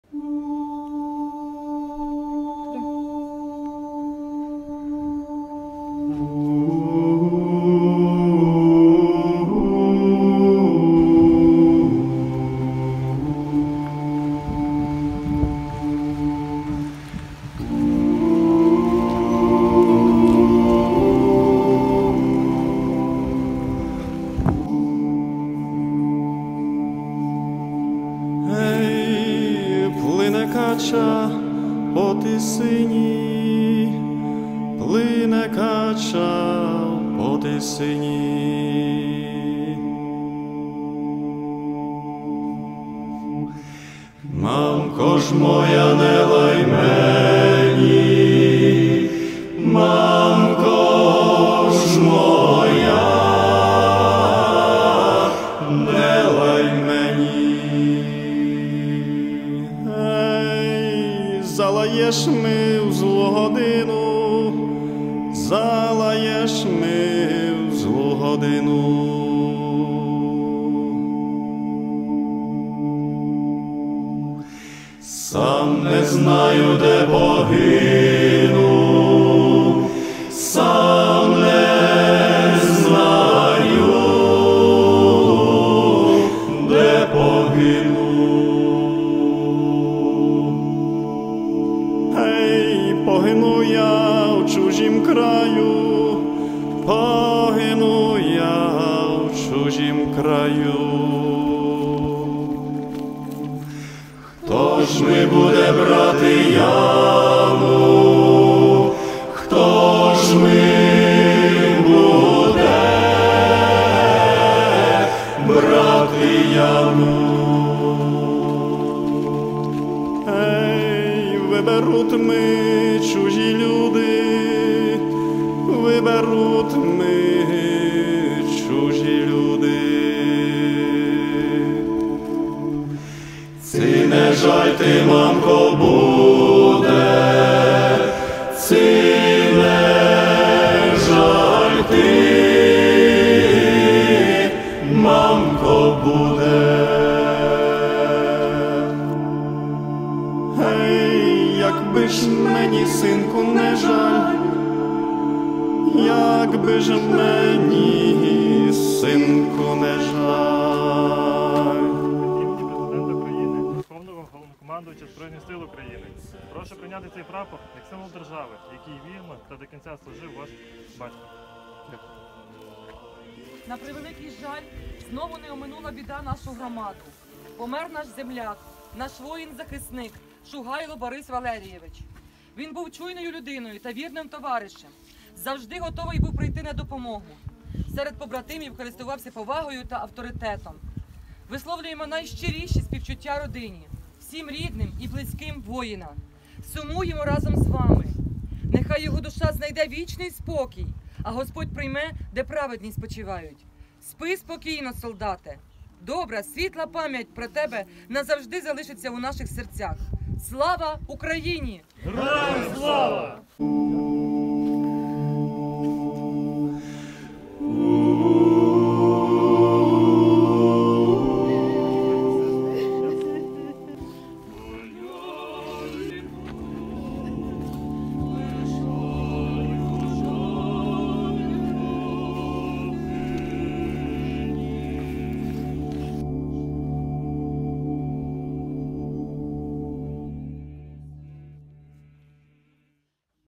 У Коростишеві віддали належну шану захиснику Борису Шугайлу
Знову невимовним болем та гірким смутком прийшла страшна звістка до Коростишівської громади. Помер Борис Валерійович  ШУГАЙЛО , 1967 року народження, військовослужбовець, який захищав нашу свободу і незалежність в лавах Збройних сил України.

Сьогодні, 25 серпня 2023 року, у Коростишеві відбулося прощання з мужнім воїном, який боронив Україну від російського агресора.

Поховання відбулося на військовому секторі міського кладовища, віддавши належну шану.

Хоробрий воїн захищав не тільки Україну, а й кожного з нас, нашу свободу і незалежність. Життя людини є безцінним, душа – безсмертна, а пам’ять про Героїв – вічна!

Коростишівська громада й уся Україна глибоко сумує і співчуває родинам кожного воїна та жодними словами не втамувати пекучий біль, не загоїти рани у душі.

Нехай опіка Всевишнього й наші молитви полегшать гіркоту втрати і родинне горе. Схиляємо голови у великій скорботі.

Вічна пам’ять та шана кожному нашому захиснику!